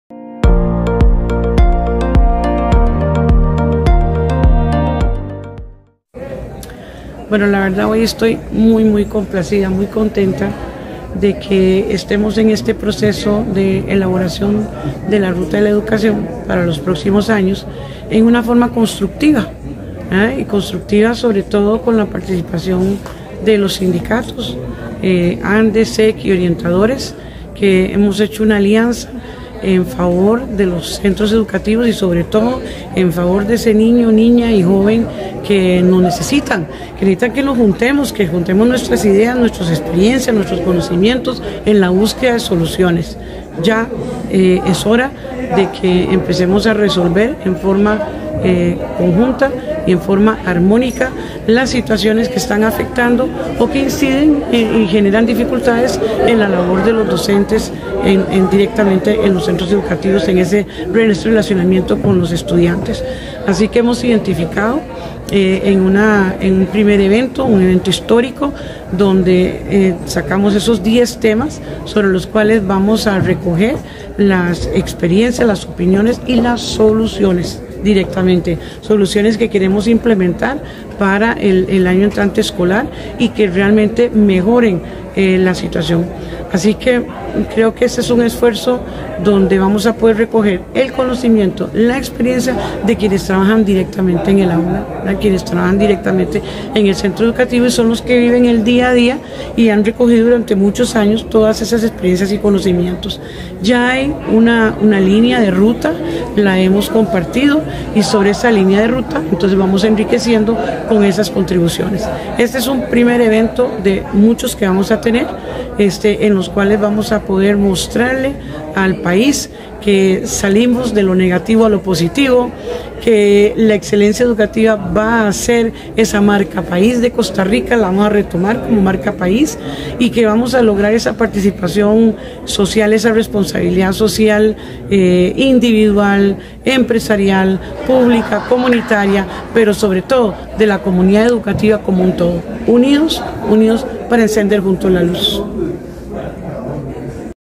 Declaración de la Ministra de Educación, Anna Katharina Müller - "Ruta de la Educación
Declaración de la Ministra de Educación, Anna Katharina Müller Castro, sobre, MEP realiza consulta nacional con 400 docentes sindicalizados para definir “Ruta de la educación"

- Ministra se reúne con docentes para trabajar acciones que refresquen e impulsen la excelencia educativa.

- Los temas por tratar los decidieron las agrupaciones gremiales de educación en conjunto con el MEP.

- Violencia, permanencia en el sistema educativo, herramientas educativas y empleabilidad figuran entre los temas en los que trabajarán las mesas